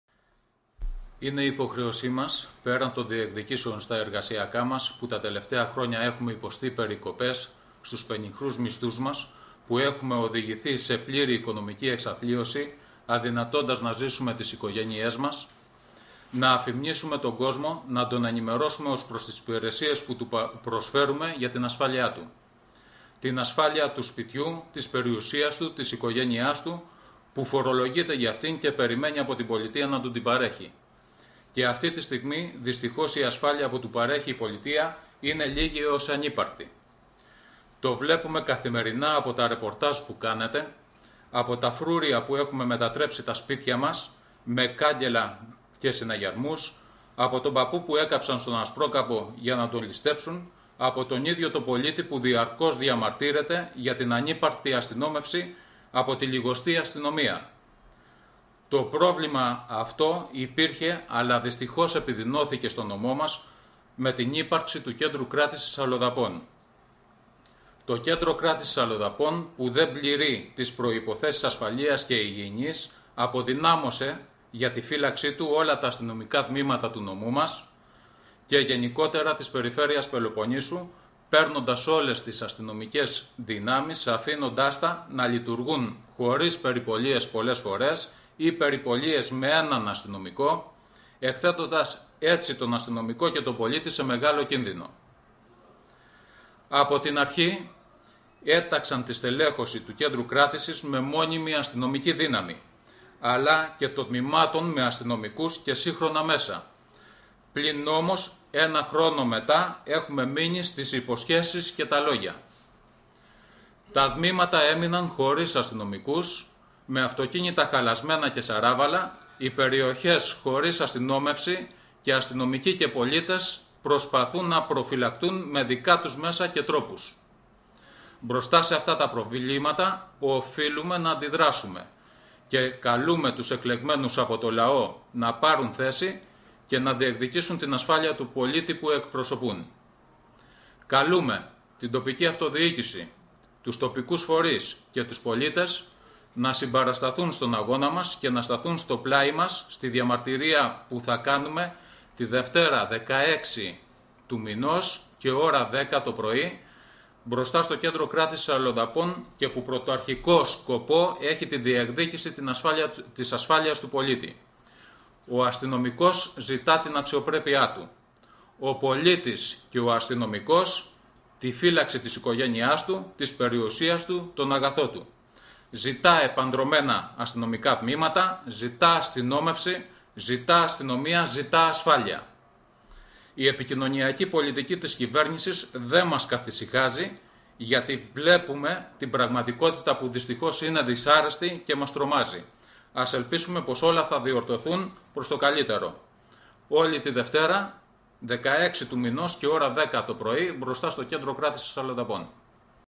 ΑΔΑΜ ΧΡΗΣΤΟΣ
Ο ΠΡΟΕΡΔΟΣ ΤΗΣ ΕΝΩΣΗΣ ΑΣΤΥΝΟΜΙΚΩΝ ΥΠΑΛΛΗΛΩΝ ΚΟΡΙΝΘΙΑΣ ΧΡΗΣΤΟΣ ΑΔΑΜ ΓΙΑ ΤΗΝ ΑΠΕΡΓΙΑ ΤΗΣ 16ΗΣ ΣΕΠΤΕΜΒΡΙΟΥ